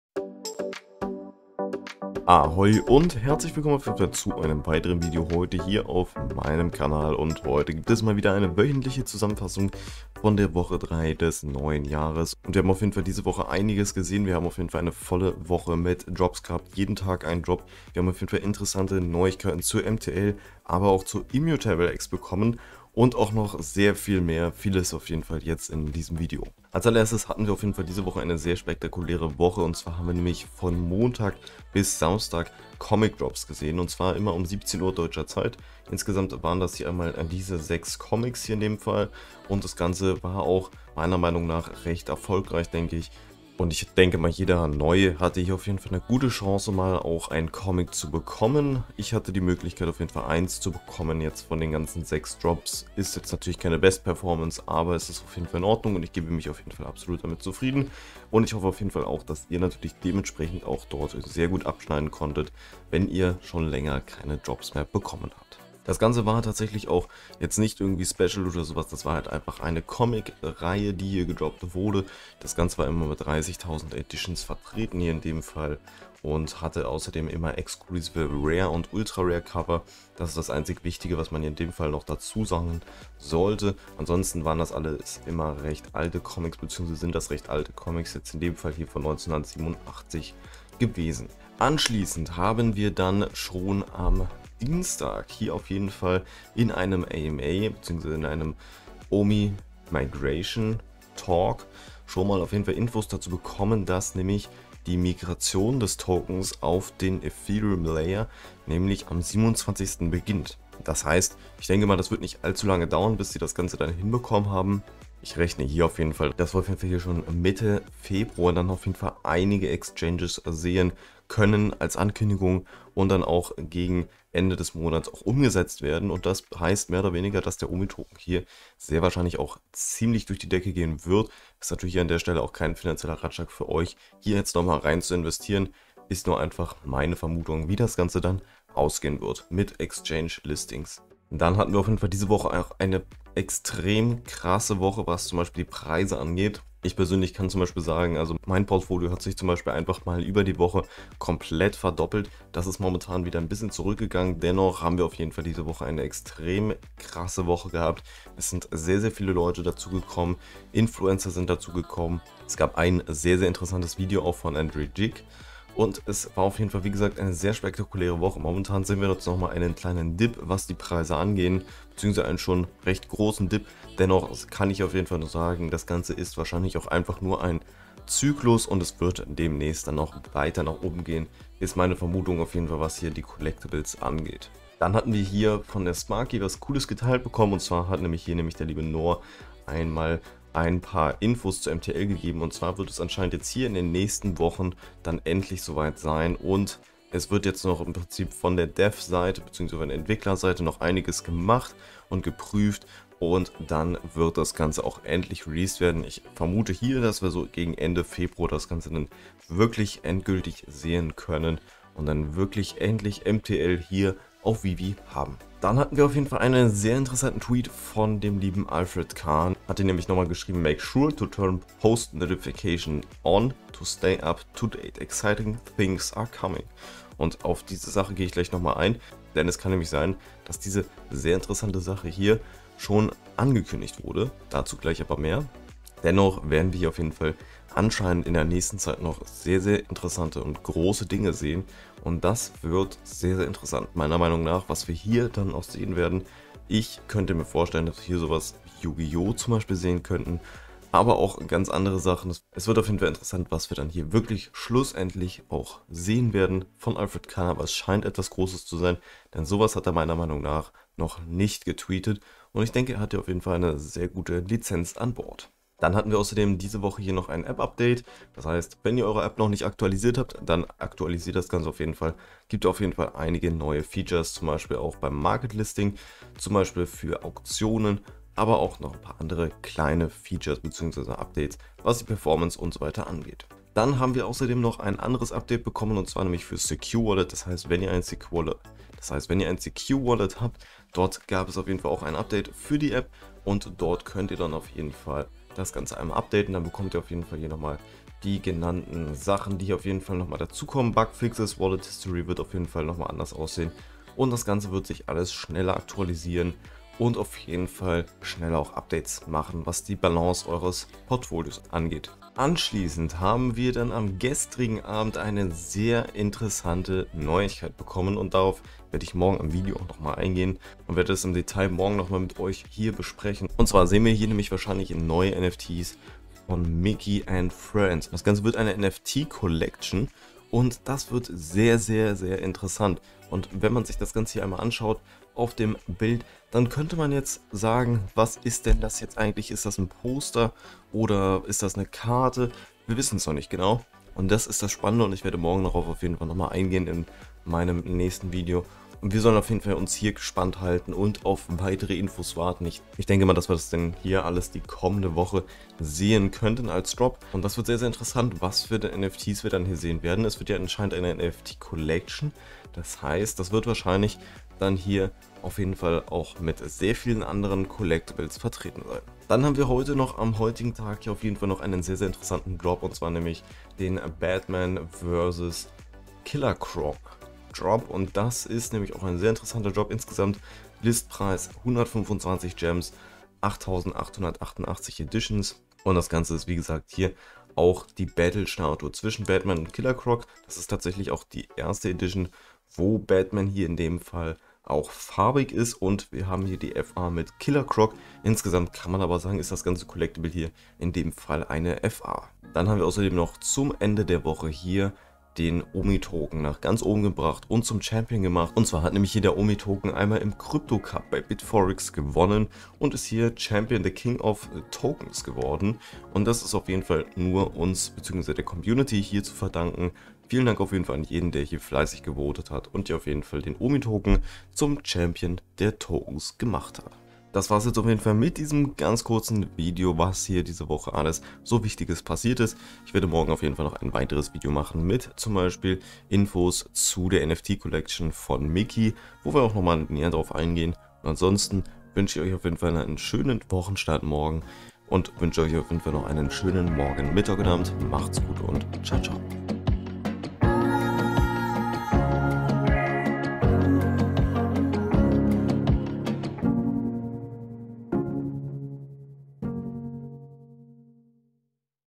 VEVE-NEWS|| Wochenrückblick #3// DISNEY DROP!!//MTL, IMX NEWS und mehr...
Diese Woche war auch wieder sehr spektakulär und voller Überraschungen. Das wichtigste ist in diesem Video zusammengefasst!

Kapitel:
00:00 Intro
00:26 Comic Drops
01:31 IMX News
02:21 Preise 🚀
03:04 MTL News
03:35 Großes kommt!
04:25 Updates!
05:12 Disney NFTs!!
06:09 DC Drop
06:42 OMI ist Champion
07:07 Outro

                   ➖➖➖➖➖➖⬇️Quellen⬇️➖➖➖➖➖➖

         ➖➖➖➖➖➖⬇️Weitere VeVe Yter⬇️➖➖➖➖➖➖